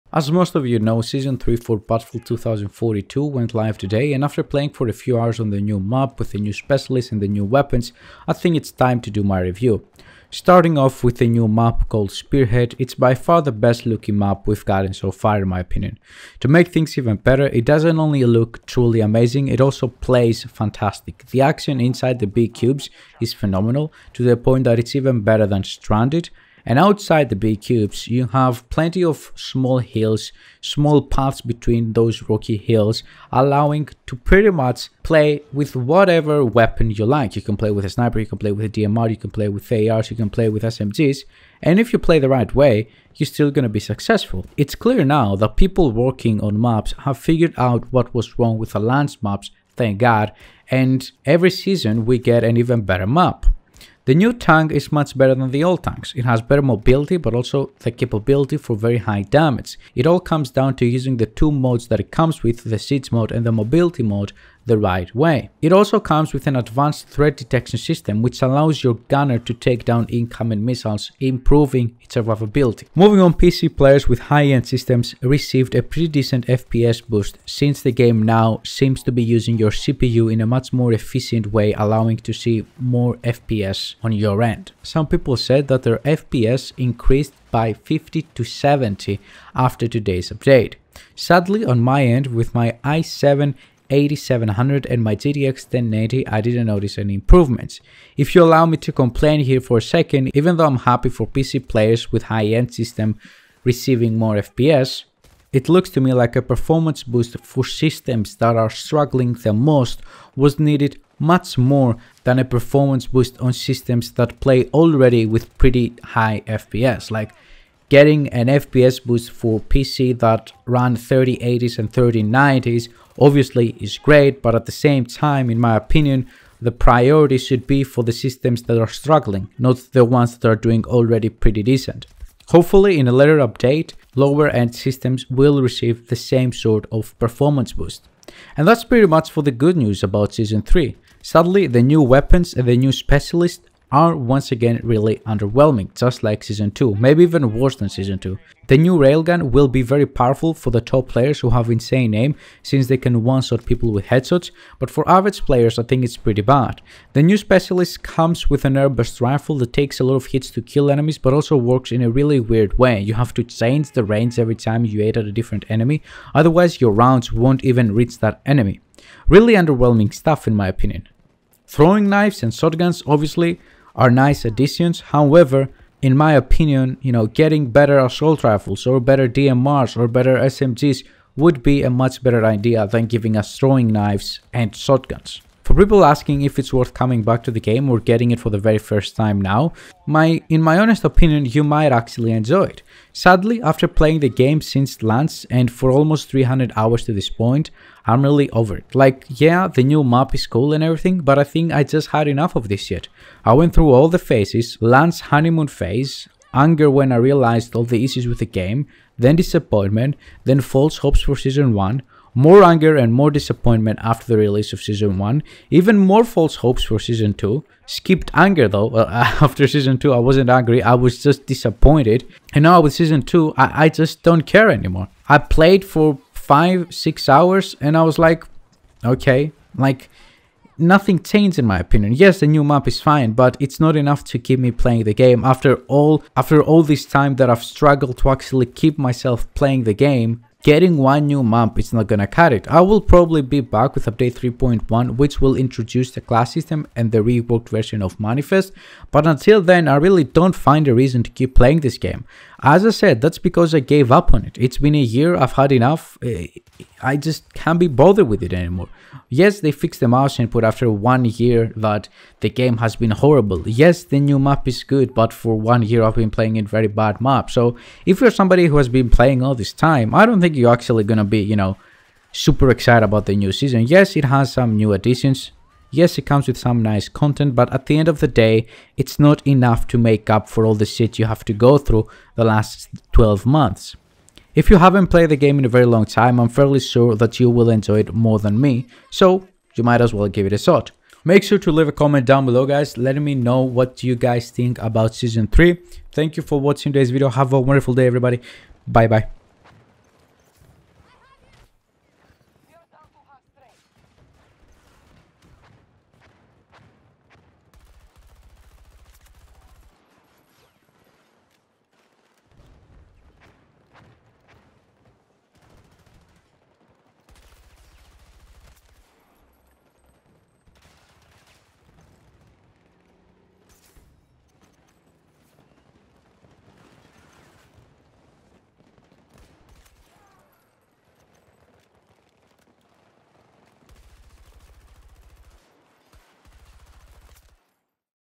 Season 3 Good Enough? Battlefield 2042.
Season 3 of Battlefield 2042 went live today. Players were excited about it from the trailers and the screenshots but is it really worth coming back to the game?